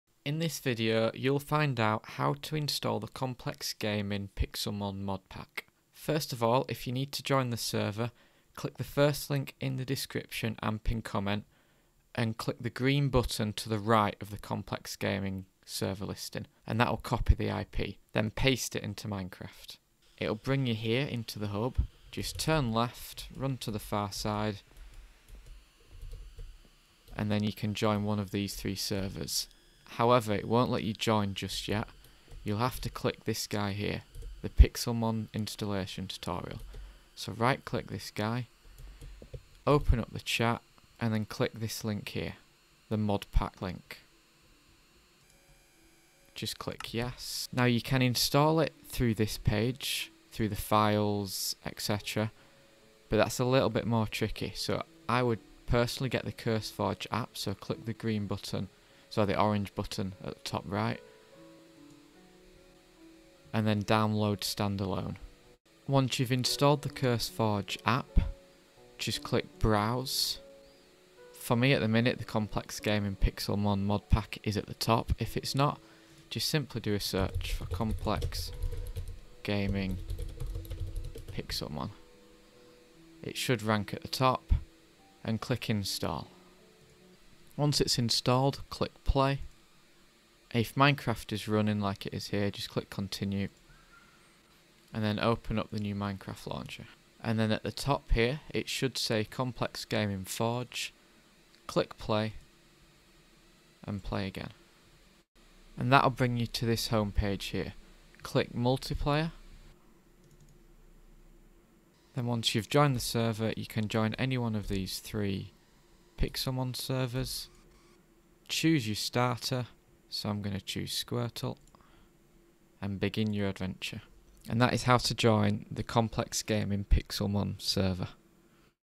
How to Install Complex Gaming Pixelmon Modpack
This video showcases How to Install Complex Gaming Pixelmon Modpack.
Copy the Complex Gaming server IP through this link ^

Tutorial on How to Install Pixelmon on Complex Gaming server.